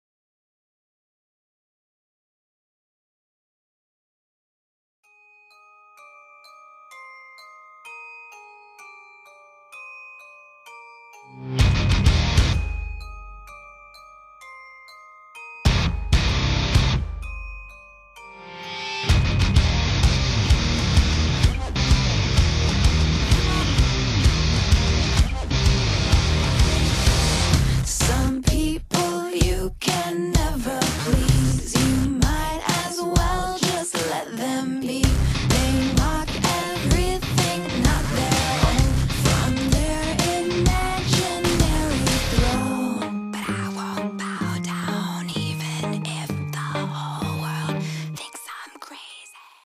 Hata mei-shin my vocaloid
1st half shes in a royal touch 2nd how she REALLY LOOKS I OWN NOTIN